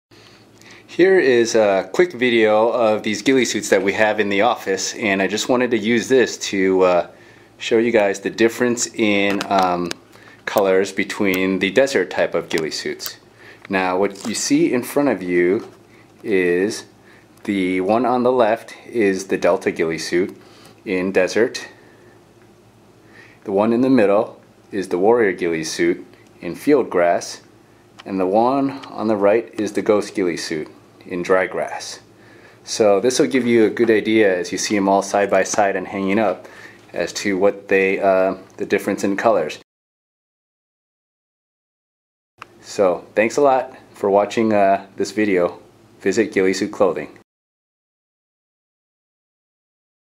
Desert Ghillie Suit Color Comparisons
compares the Arcturus Camo desert Delta Ghillie Suit vs. the field grass Warrior ghillie suit vs. the dry grass Ghost Ghillie suit.  It shows the 3 ghillie suits side by side so you can see their subtle color differences.  It does not do a ghillie suit review.